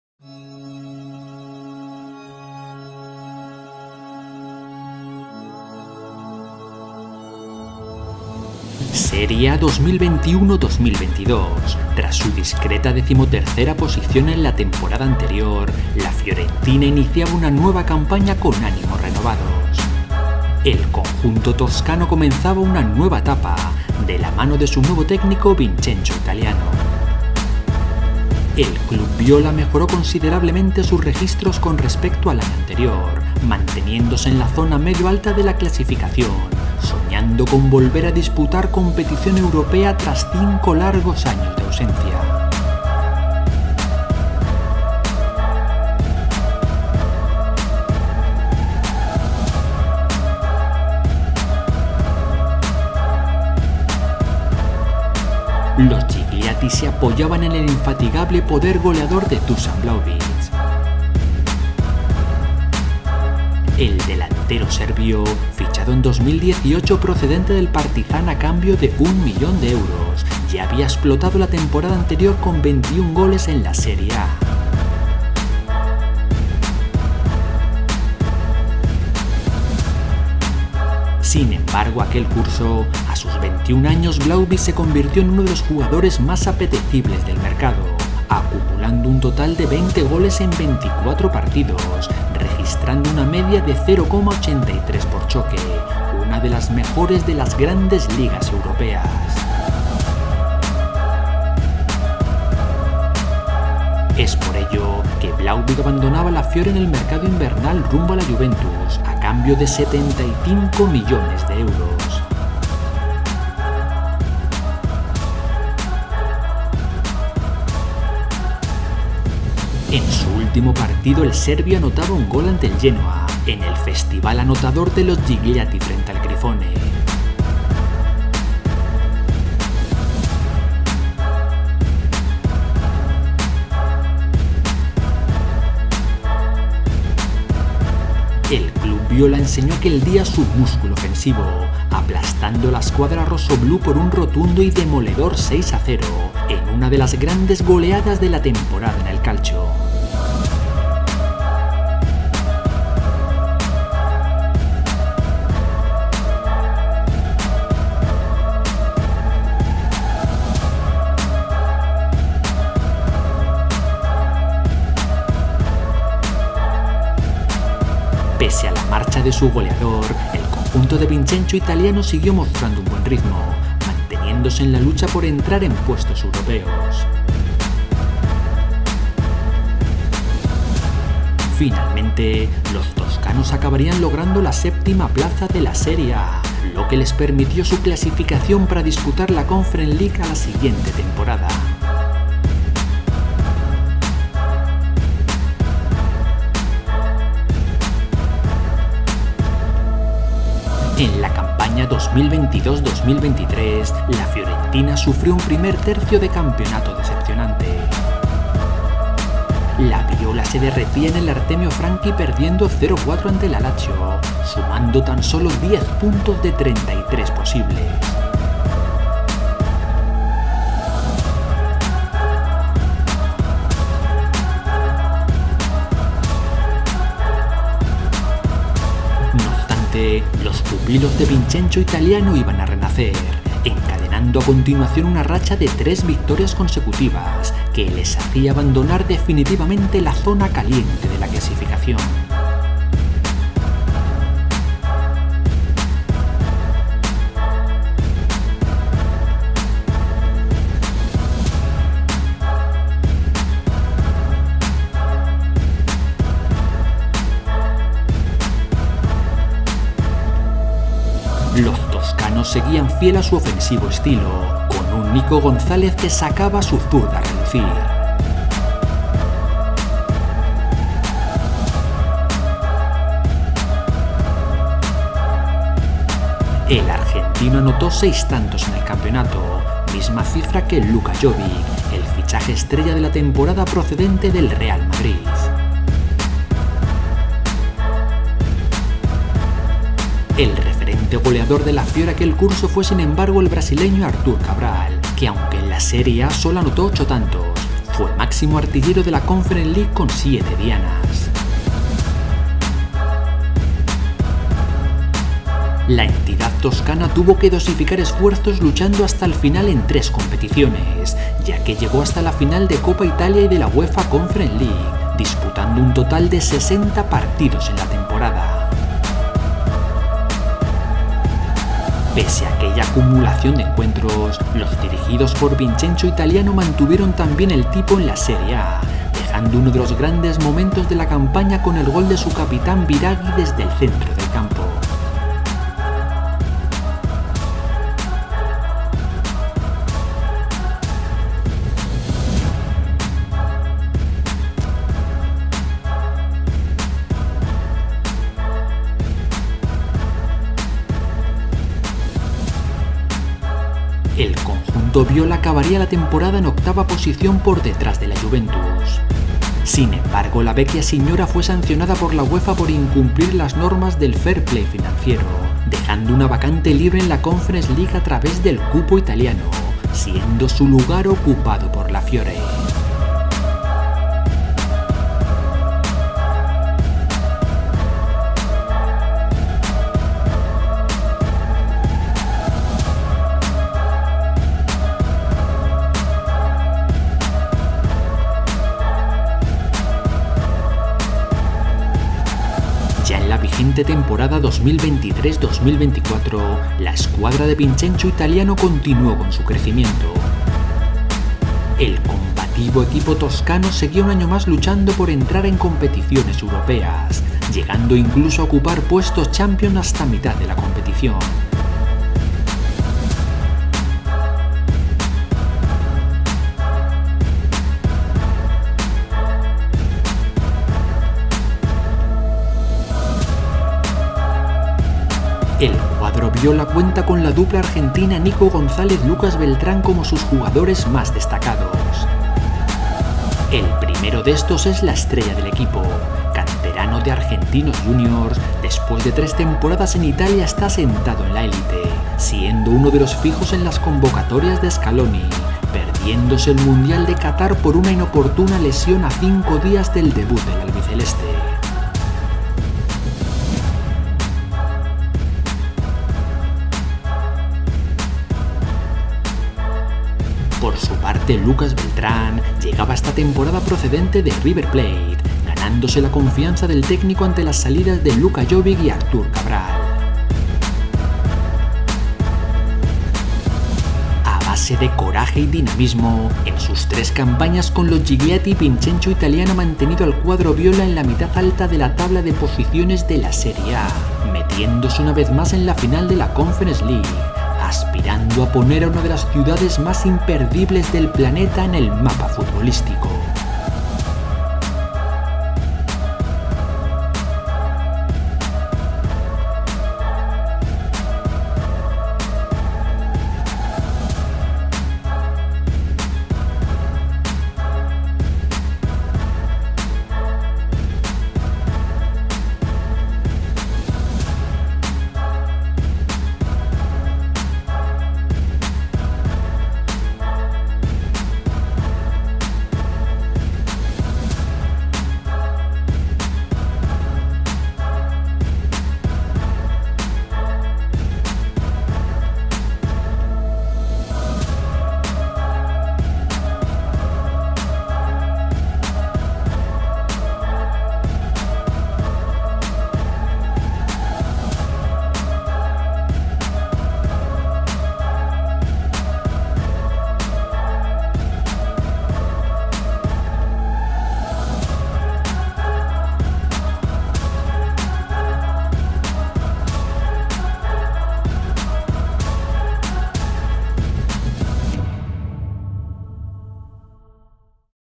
El RENACER de la FIORENTINA 💜 ᴴᴰ
▶️ A base de coraje y dinamismo, en sus 3 campañas con los gigliati Vincenzo Italiano ha mantenido al cuadro viola en la mitad alta de la tabla de posiciones de la Serie A, metiéndose una vez más en la final de la Conference League, aspirando a poner a una de las ciudades mas imperdibles del planeta en el mapa futbolístico.

📌 ÍNDICE:
0:08 Temporada 2021/2022
3:21 Temporada 2022/2023
6:33 Temporada 2023/2024

✅ The rights to use the footage contained herein were granted through our partnership with Infront Sports and WSC Sports